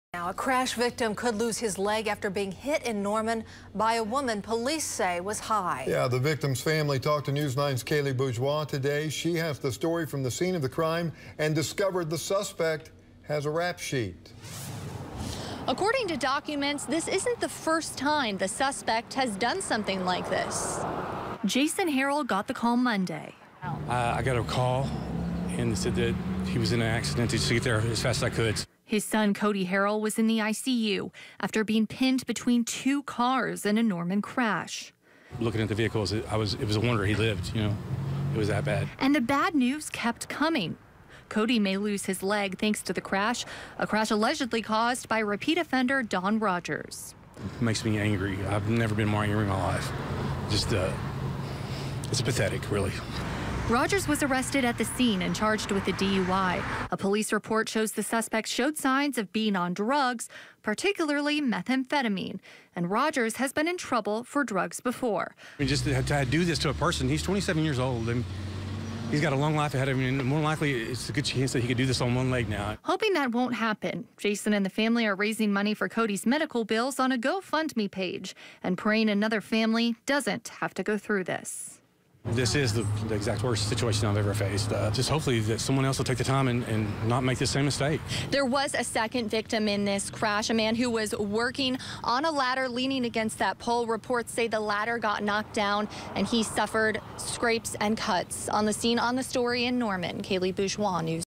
Norman Man May Lose Leg After Repeat DUI Suspect Hit Him
ᐸpᐳA local man may lose his leg after being hit by a woman who was allegedly driving high.ᐸ/pᐳ  ᐸpᐳᐸ/pᐳ